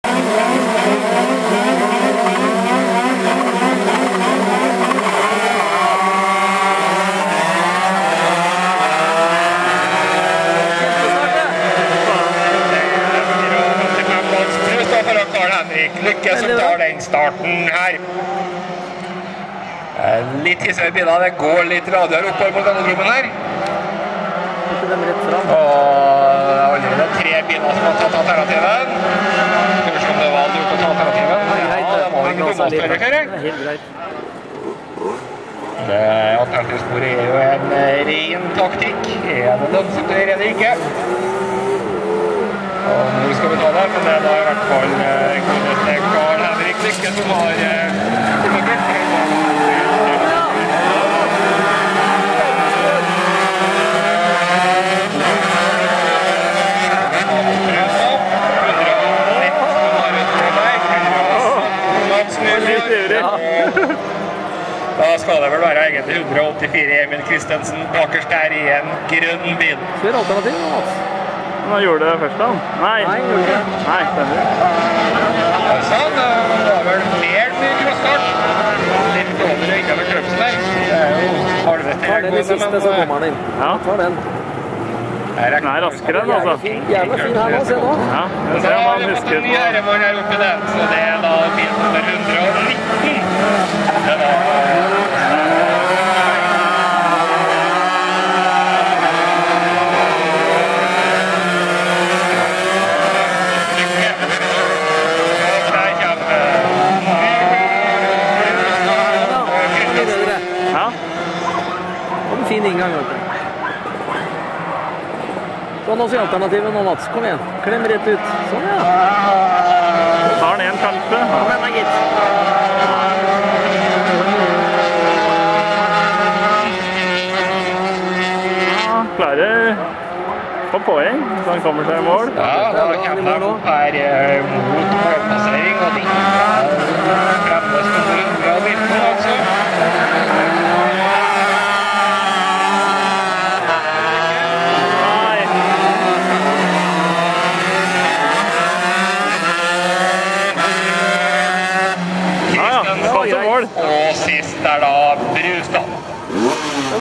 Crosskart NM Orkla 125 heat 1 - NorCart
Crosskart NM, Orkla.
125 HEAT 1.

Mitt aller første crosskartløp med NorCart VR5. Jeg starter i 2. startspor, men klarer å gjøre 2 store feil. Ender her på 4. plass..